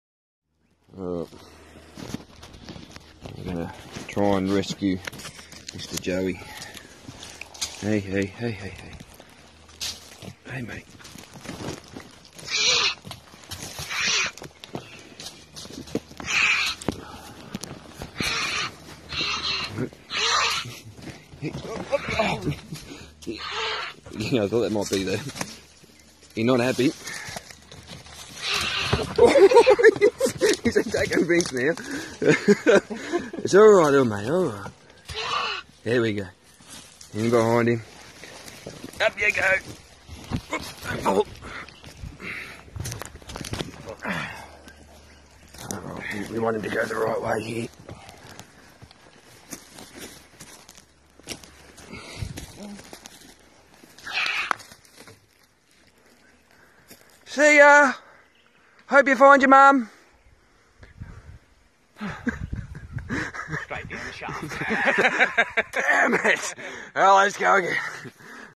Vince getting attacked by a Joey
This little fella was stuck in a old working. Vince went in to save him but not before getting attacked by it 😂